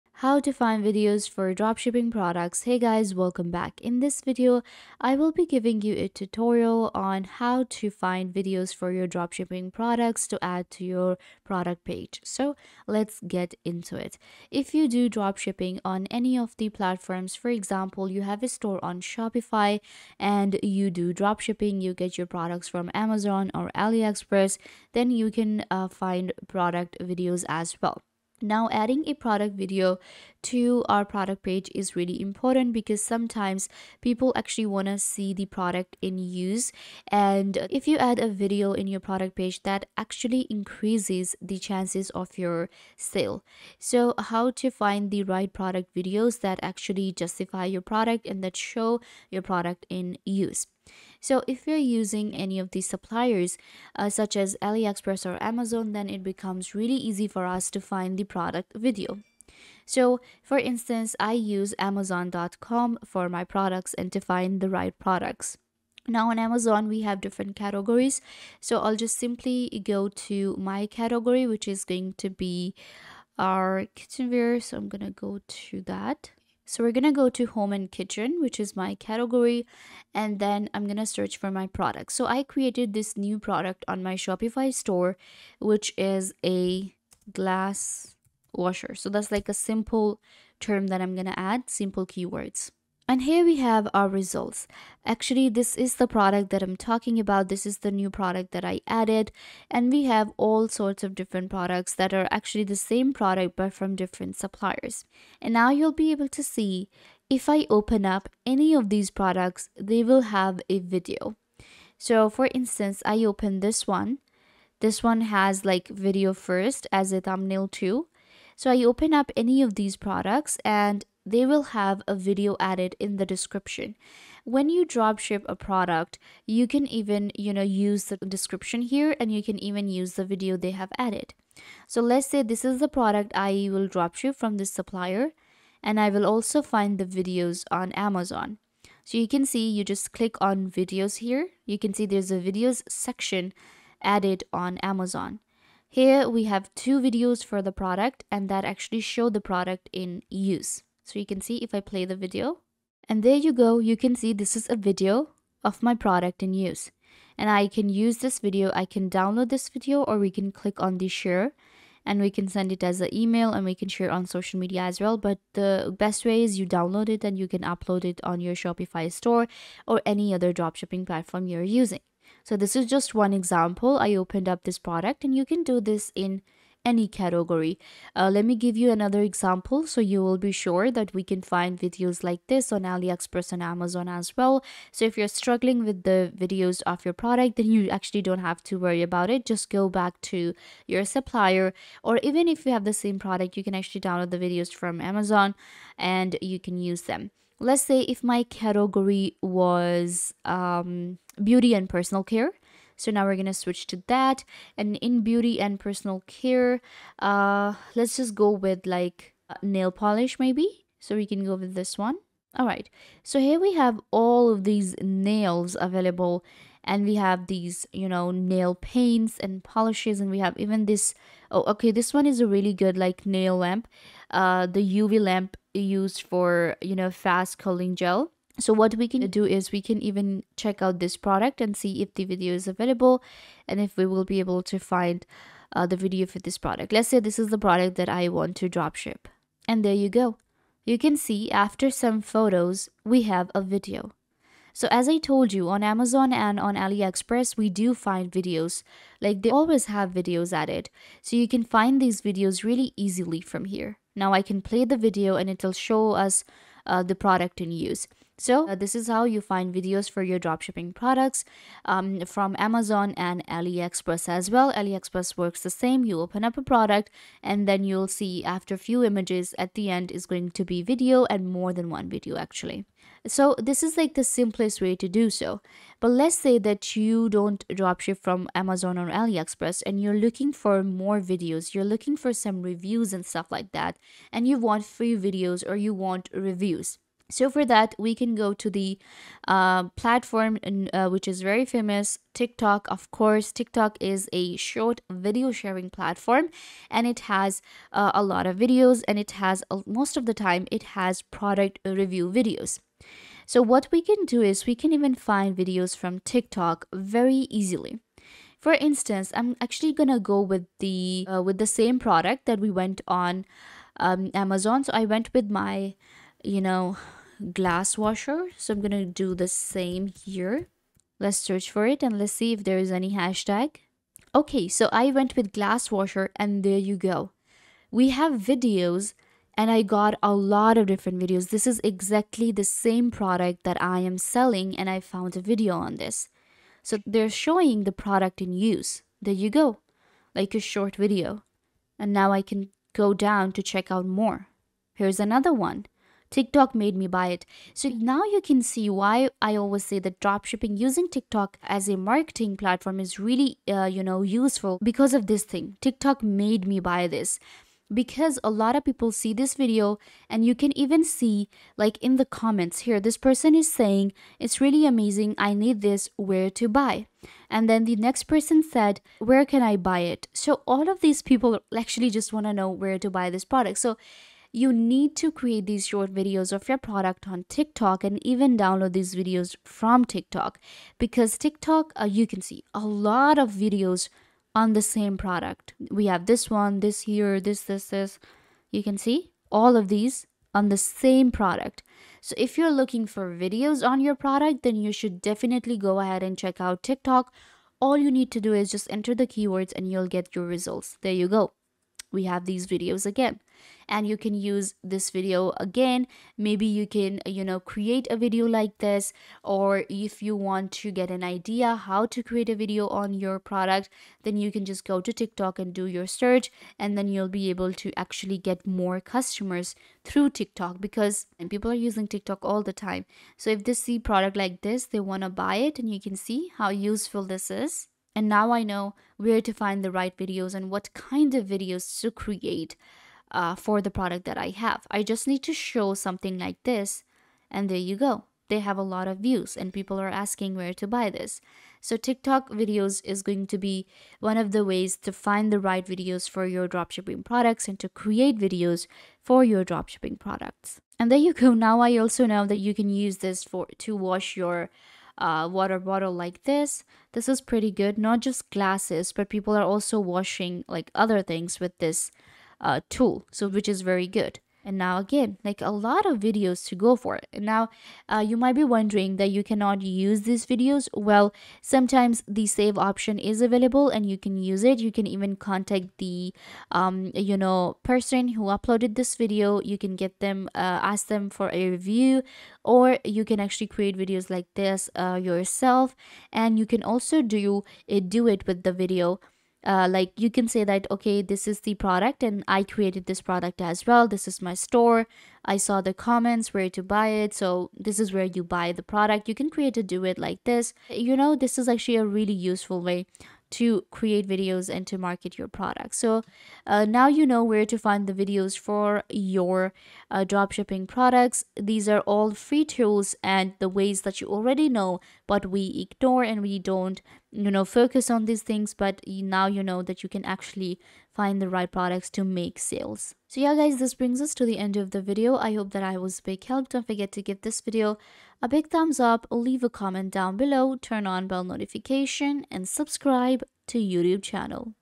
How To Find Videos For Dropshipping Products 2024
In this video we show you How To Find Videos For Dropshipping Products. Watch this video to learn to easily find videos for dropshipping products.